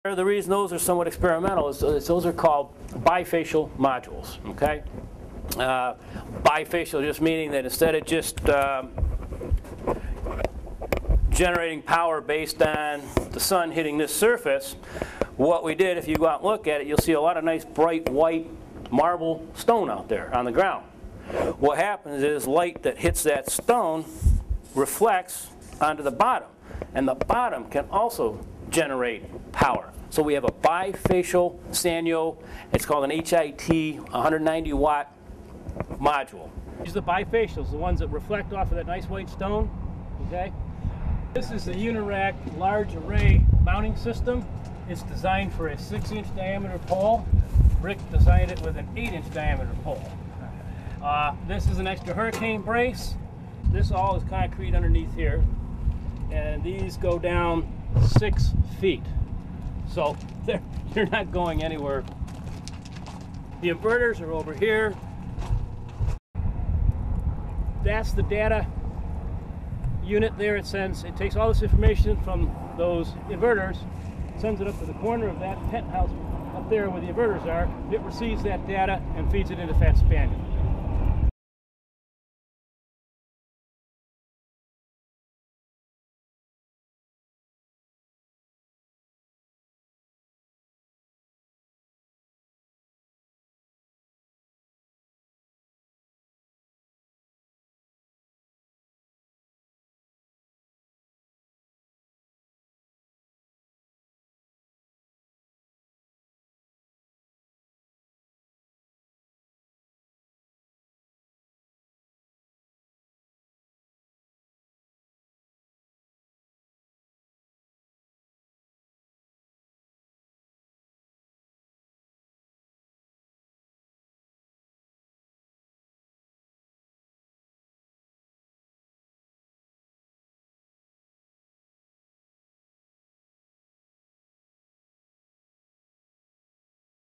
Solar PV System Installation Experimental System 3
Orange County Convention Center Solar PV System Installation Experimental System by Solar Source.  Bifacial Modules Collect Solar Energy from Both Sides.